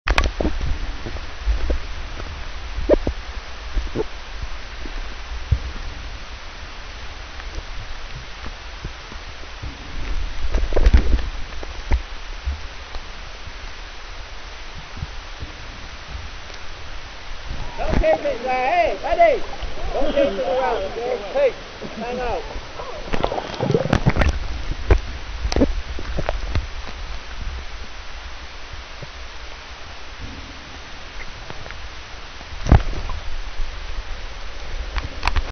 Snorkelling in Barbados
Me snorkeling with Sea Turtles in Barbados (Sept 07).